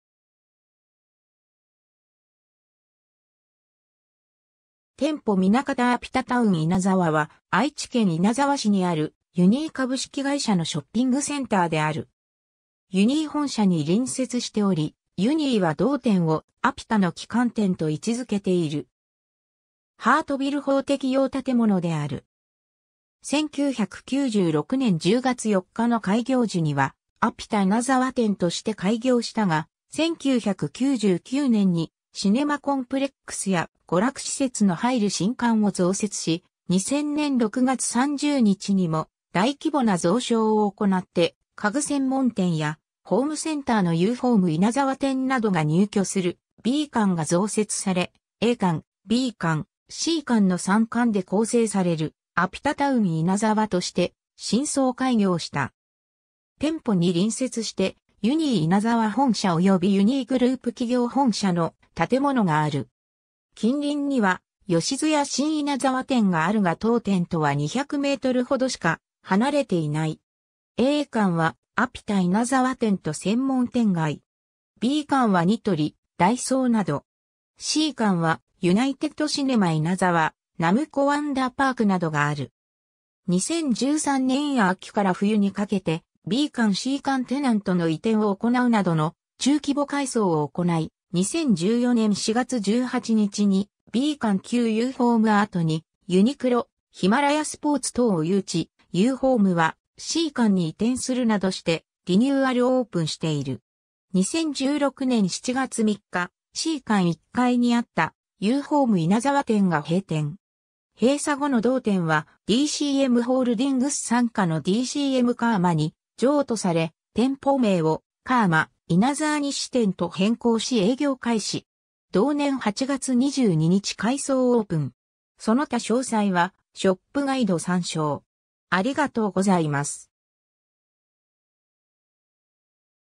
アピタタウン稲沢
店舗南方（C館） アピタタウン稲沢（アピタタウンいなざわ）は、愛知県稲沢市にあるユニー株式会社のショッピングセンターである。
ユニー本社に隣接しており、ユニーは同店をアピタの旗艦店と位置付けている。
ハートビル法適用建物である。
 1996年（平成8年）10月4日の開業時には「アピタ稲沢店」として開業したが、1999年（平成11年）にシネマコンプレックスや娯楽施設の入る新館（C館）を増設し、 2000年（平成12年）6月30日にも大規模な増床を行って家具専門店やホームセンターのユーホーム稲沢店などが入居するB館が増設され、A館・B館・C館の3館で構成される「アピタタウン稲沢」として新装開業した。
 店舗に隣接してユニー稲沢本社およびユニーグループ企業本社の建物がある。
 近隣にはヨシヅヤ新稲沢店があるが当店とは200メートルほどしか離れていない。
 A館はアピタ稲沢店と専門店街。
B館はニトリ（家具）、ダイソーなど。
C館はユナイテッド・シネマ稲沢（映画館　9スクリーン、2,696席）、ナムコ・ワンダーパークなどがある。
 2013年（平成25年）秋から冬にかけて、B館C館テナントの移転を行うなどの中規模改装を行い、2014年（平成26年）4月18日にB館旧ユーホーム跡に、ユニクロ、ヒマラヤスポーツ等を誘致、ユーホームはC館に移転するなどしてリニューアルオープンしている。
 2016年（平成28年）7月3日、C館1階にあったユーホーム稲沢店が閉店。
閉鎖後の同店はDCMホールディングス傘下のDCMカーマに譲渡され、店舗名を「カーマ稲沢西店」と変更し営業開始。
同年8月22日改装オープン。
 その他詳細はショップガイド参照。